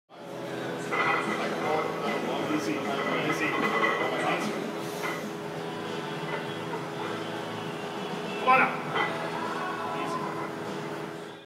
Nov 7 squat
525 and 555 singles. Beats my gym PR of 550! Definitely gunning for over 600 at nationals!